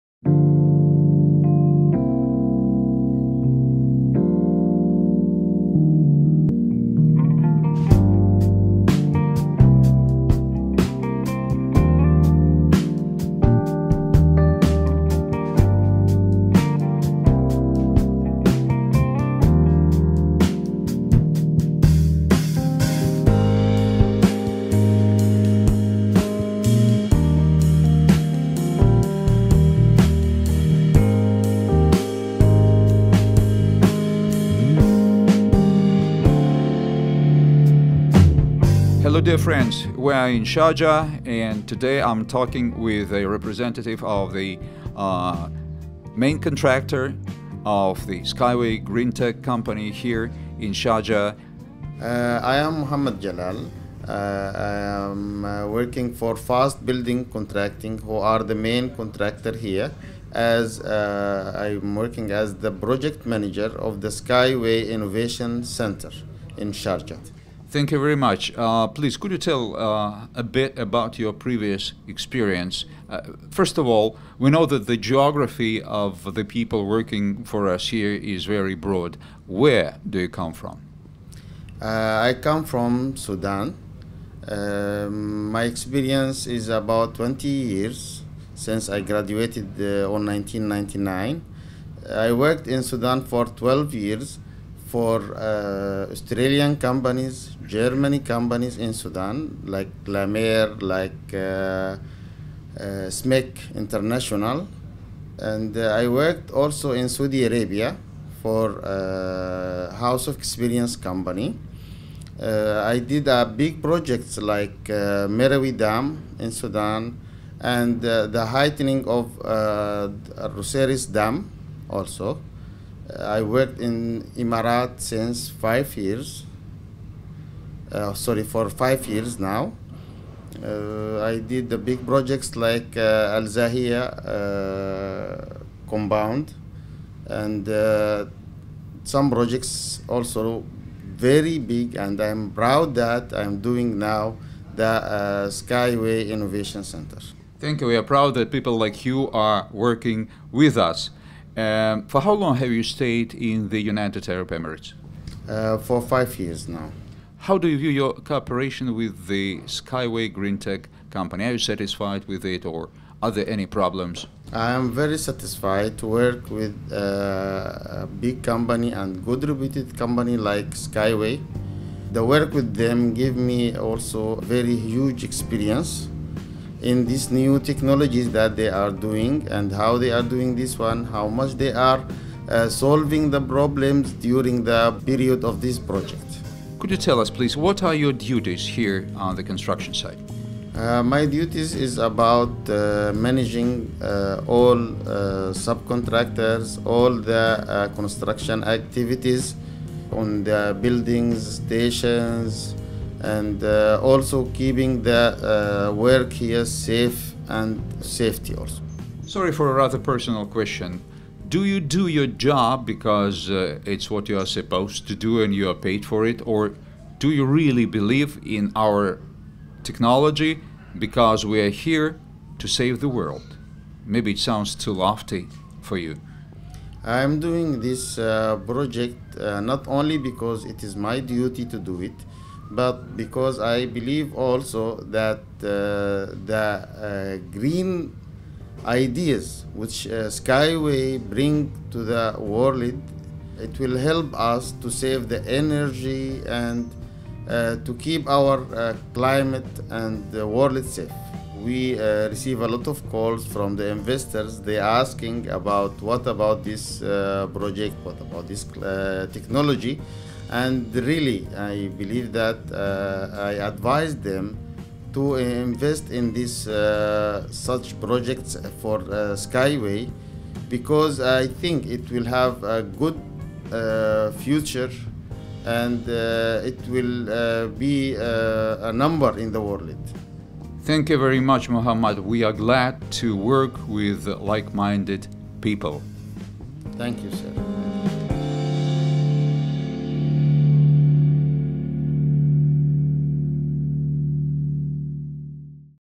Interview with Mohammed Galal
Our partners have already got used to publications about key employees of the company-developer of Unitsky String Transport technology and of the entire SkyWay Group of Companies, but interviews with representatives of partner organizations do not often appear on our resources. Today’s conversation is one of such rare opportunities. We invite you to meet Mohammed Galal, Projects Manager of Fast Building Contracting, the main contractor of SkyWay GreenTech in Sharjah.

The degree of our interlocutor’s responsibility is hard to overestimate: his duties include managing the activities of all subcontractors doing work at the construction site, and ensuring their safety. It is known that representatives of more than 20 countries are working on implementation of our project at SkyWay Innovation Center. Mohammed is a native of Sudan and has been working successfully in the UAE for five years.

But let’s not run ahead – we will give the floor to Mr. Galal himself.